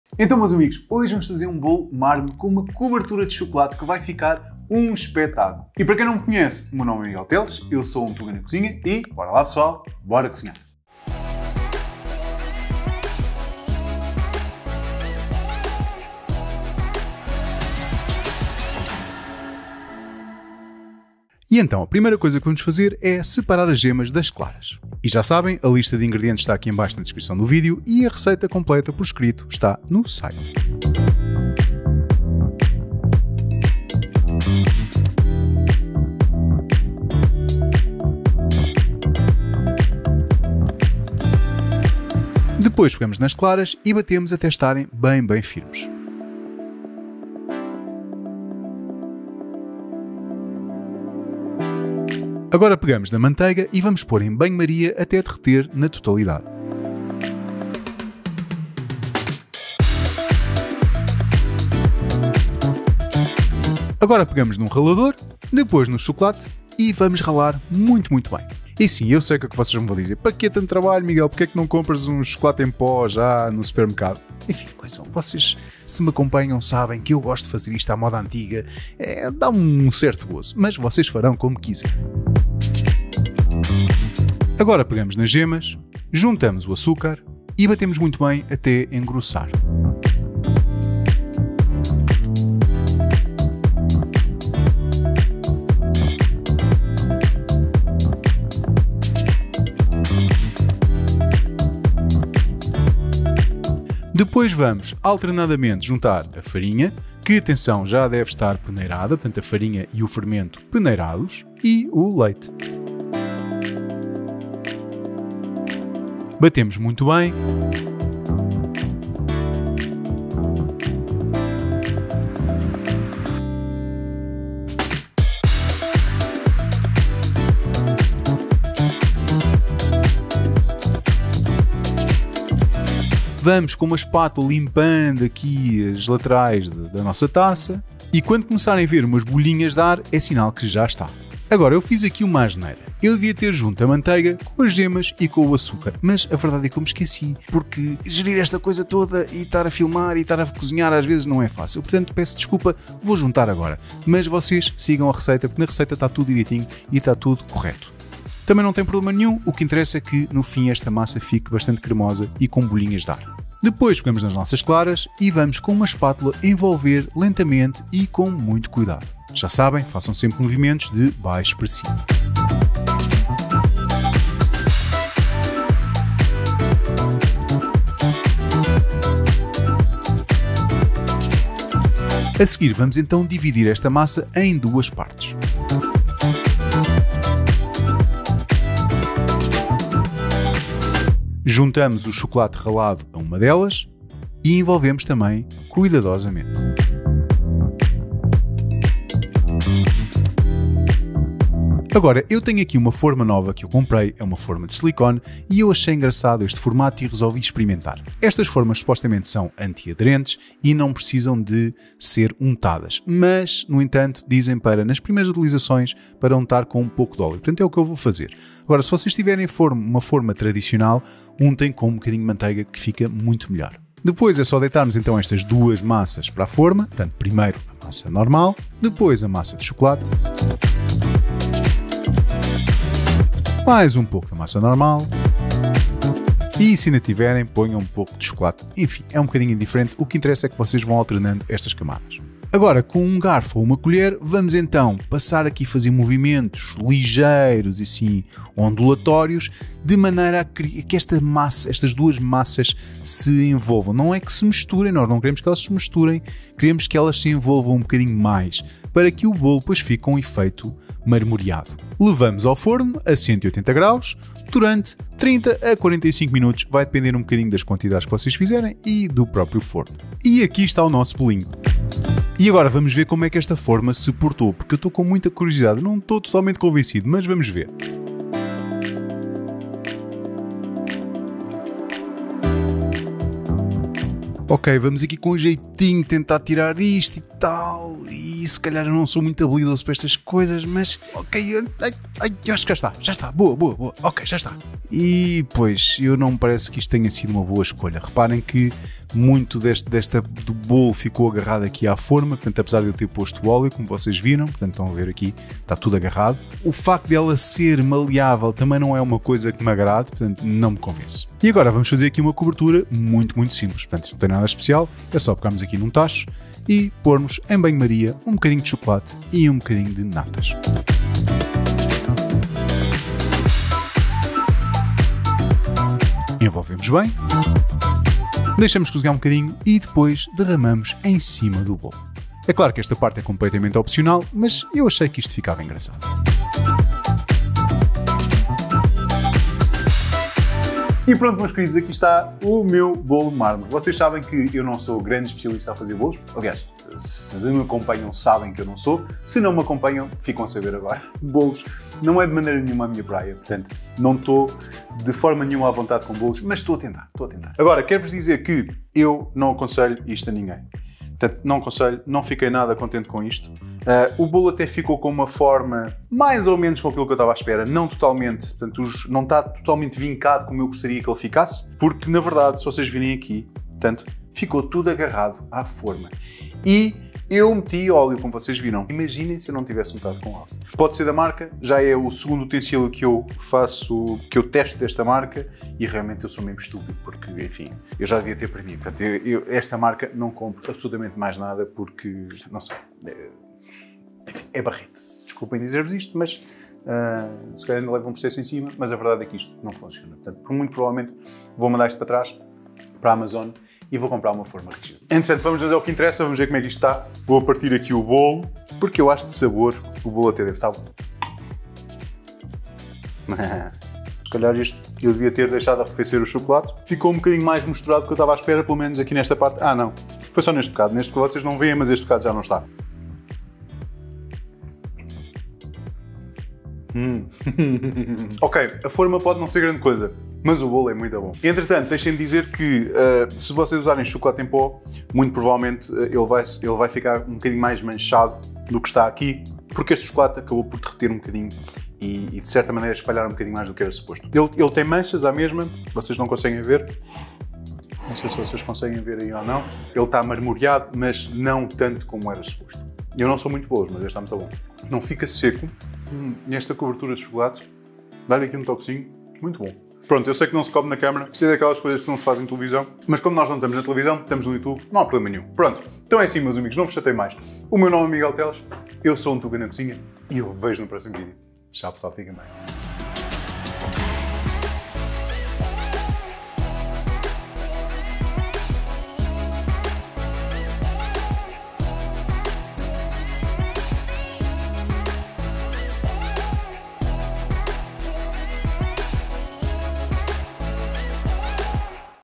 Bolo Mármore à antiga | super fofinho!
E que tal um Bolo Mármore como se fazia antigamente? ;)... Este bolo fica super fofinho, vale mesmo a pena experimentar :)...

Bora lá pessoal, bora cozinhar ;)...

Faca que uso
Ingredientes

375g farinha
9g Fermento em pó
5 Ovos
120g Chocolate em barra
375g de açúcar
300g de manteiga
300ml de Leite

Cobertura
150g de chocolate em barra
150ml de natas

00:00 - Intro
00:20 - Receita
05:41 - Segmento final